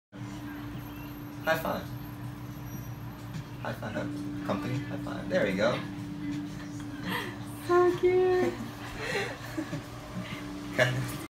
Percy Big Ear Yorkie Giving High Five
Puppy giving high five